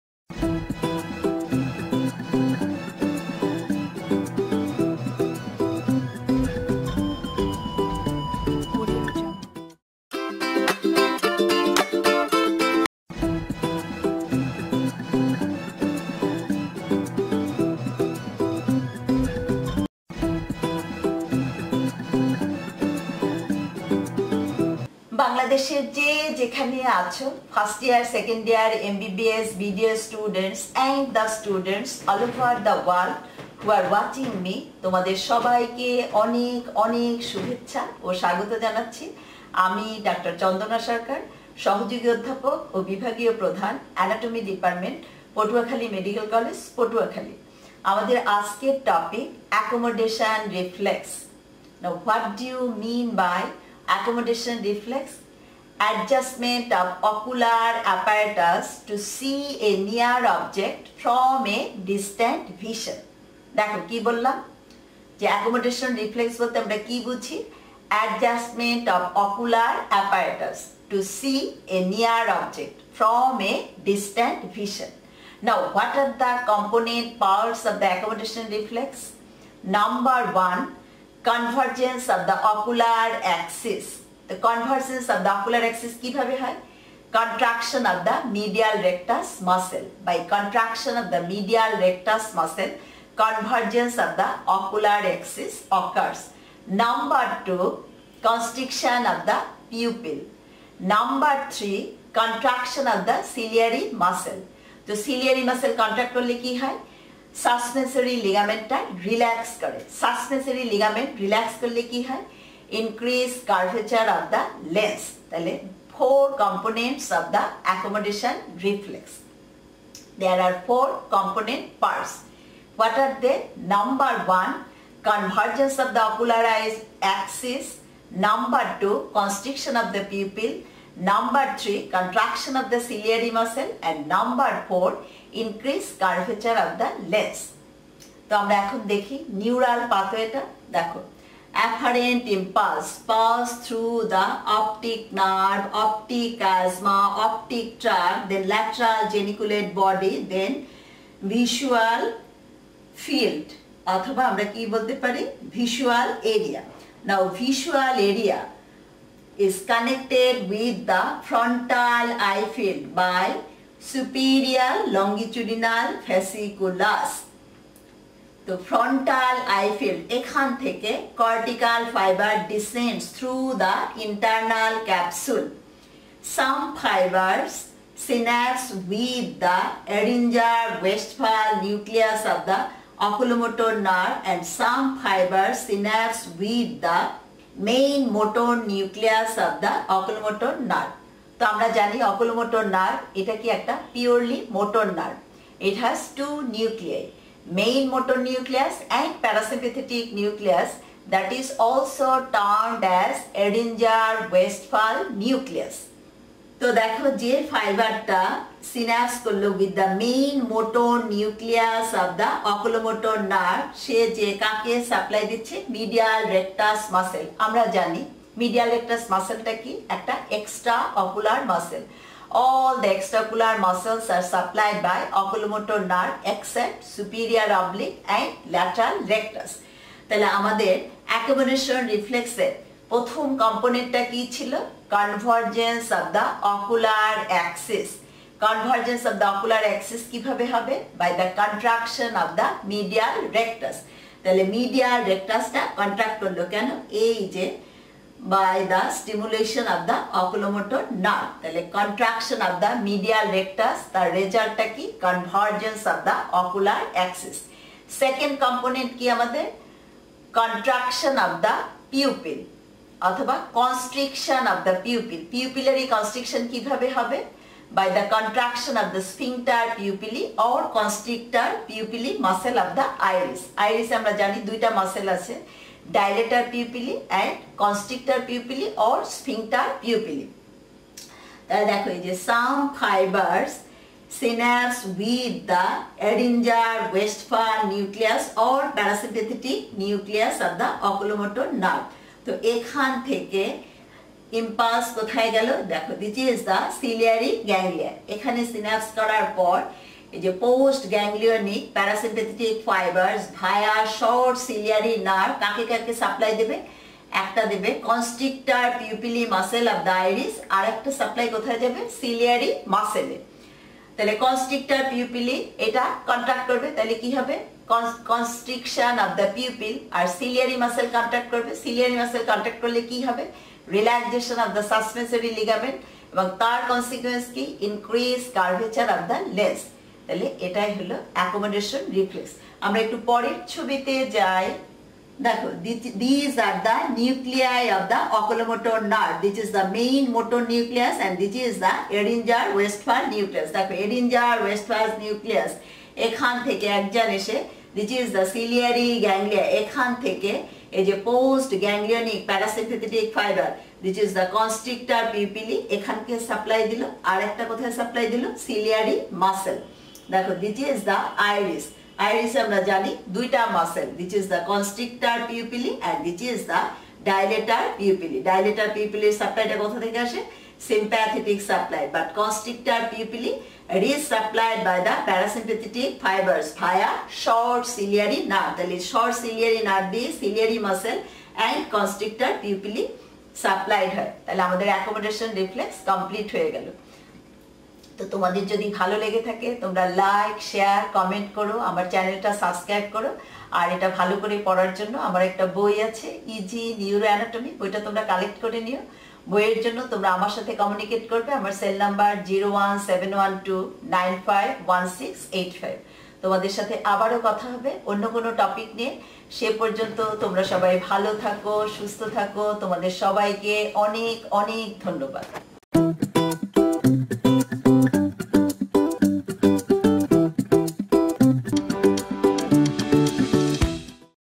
Lecture On Accomodation Reflex
In this lecture definition, component parts and the neural pathway of accomodation reflex have been described.